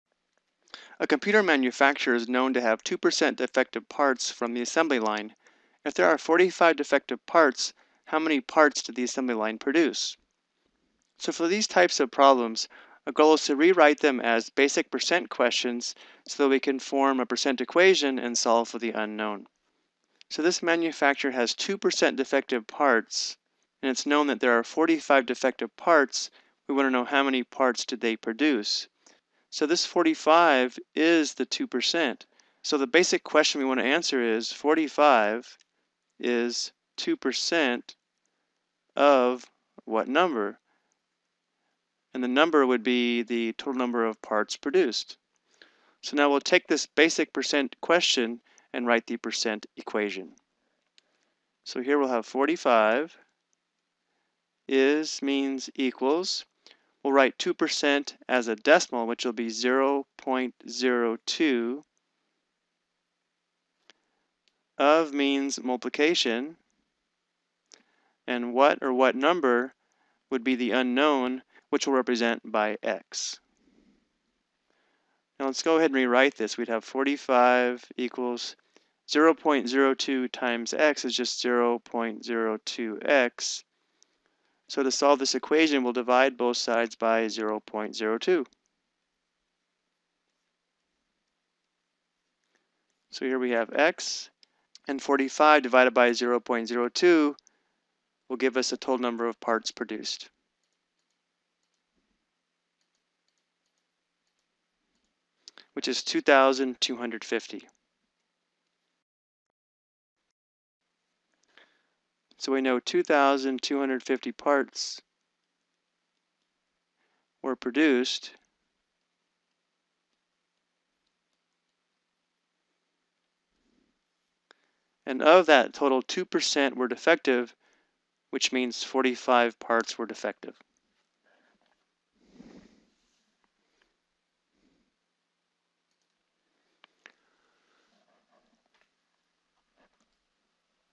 Example 3: Percent Application Problem
This video determines if 2% of  items are defective and there are 45 defective items, how many items were produced.